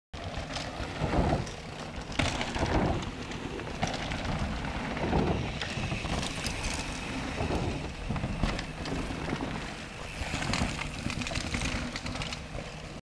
Jump area - MTB park Javornik
Friends hit last jumps in bikepark Jvornik.